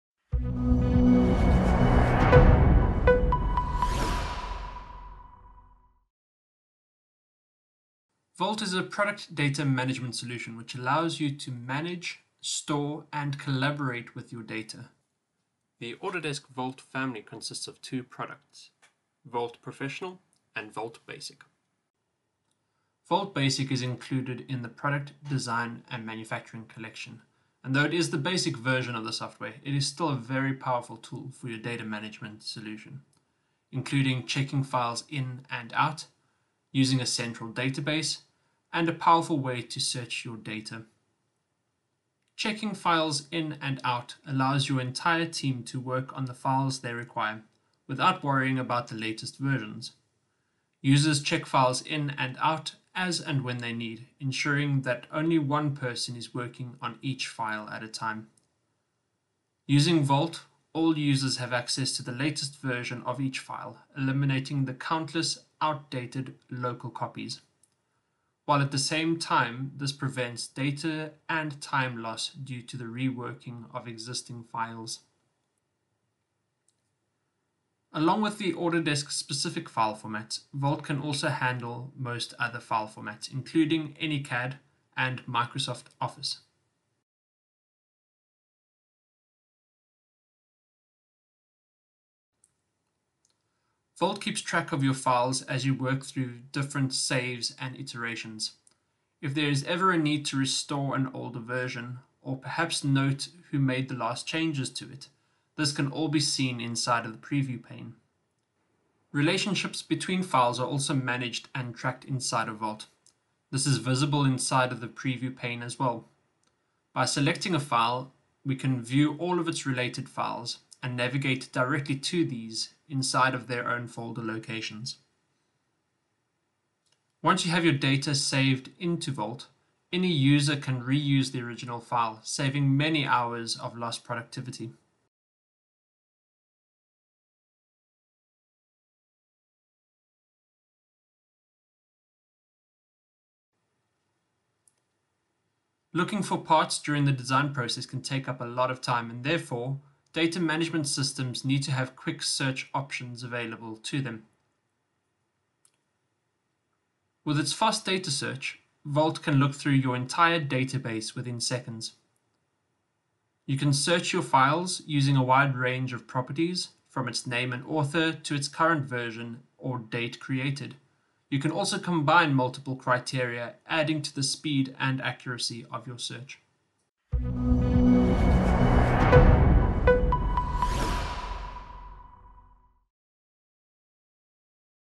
Autodesk Vault Basic Overview
In this video we look at some of the functionality and benefits of Autodesk Vault Basic. Vault is a powerful file management system which allows centralised data management. Users check files in and out with their individual accounts, search files in the database using various properties & criteria, and manage the entire database all from one program.